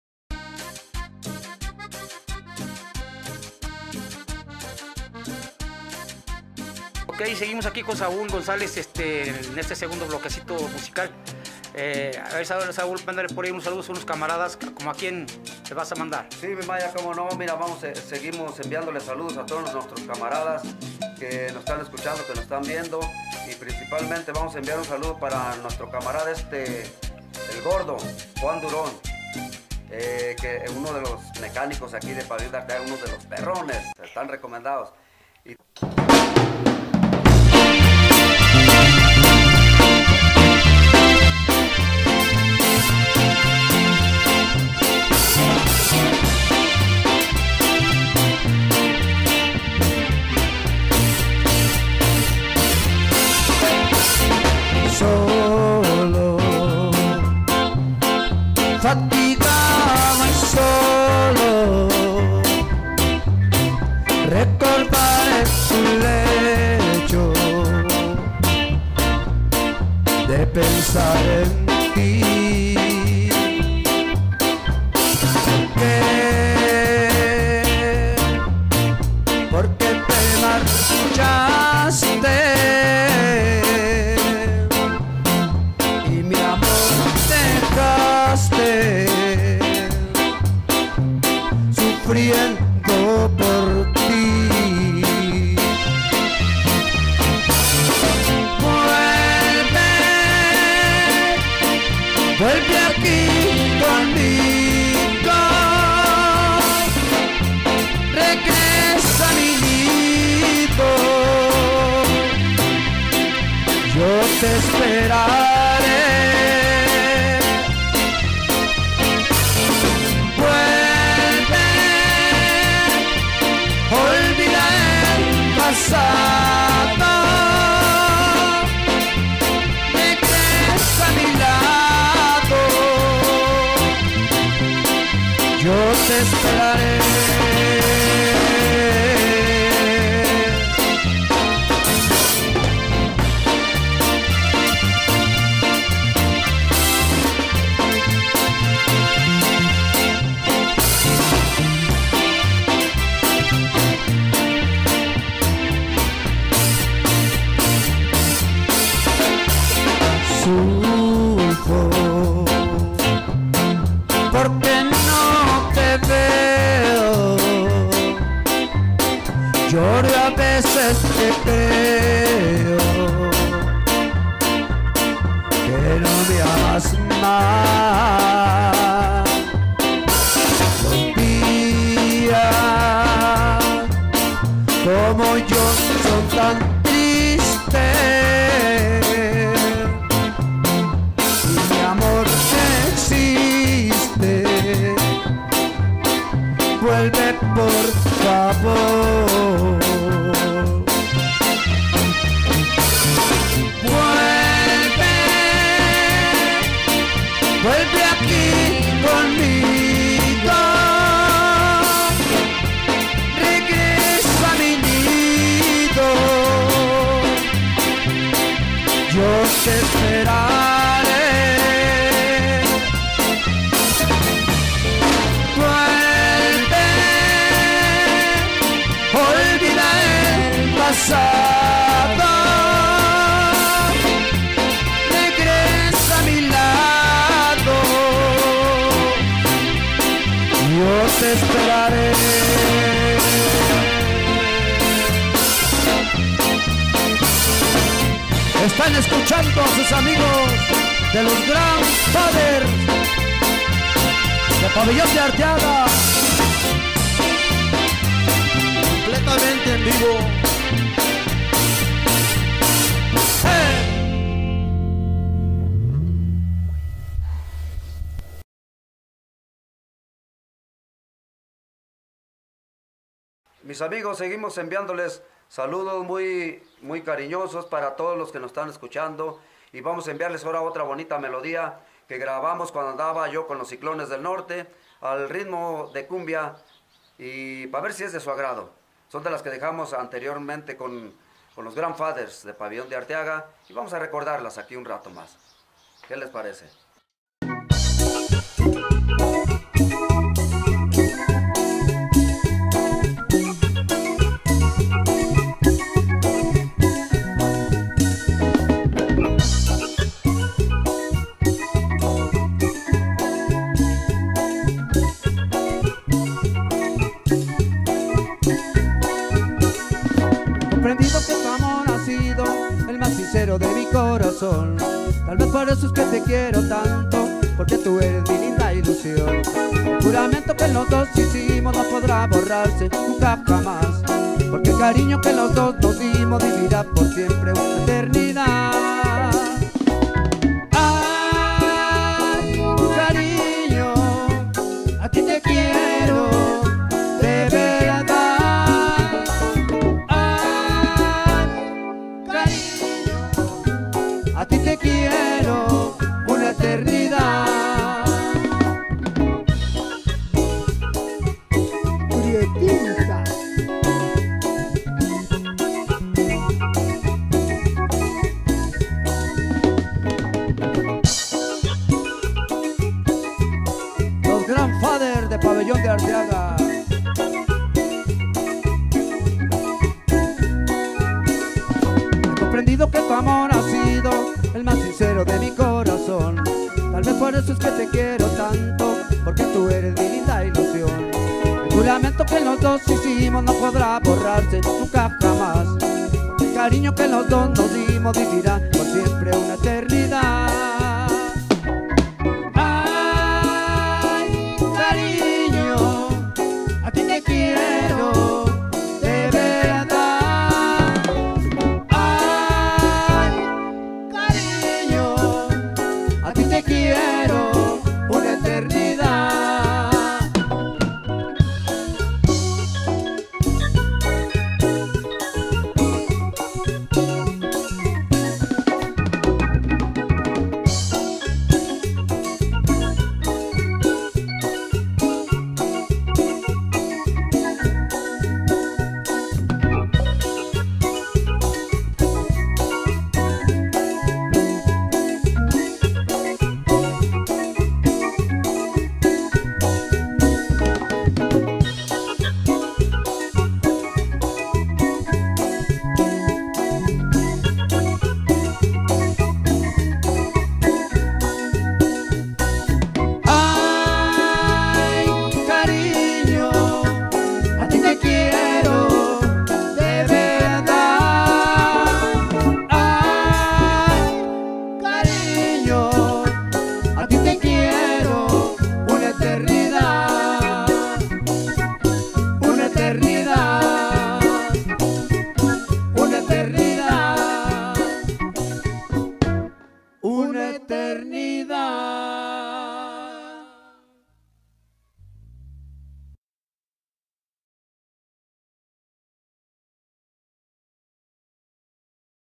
EL GRAN FHATER´S SAÚL GONZÁLEZ ARTISTA ECLUSIVO DE PABELLÓN DE ARTEAGA, AGS
SOLO ÉXITOS BONITOS CON NUESTRO ARTISTA DE PALO ALTO BARRIO DE PABELLÓN DE ARTEAGA, AGS. SAÚL GONZÁLEZ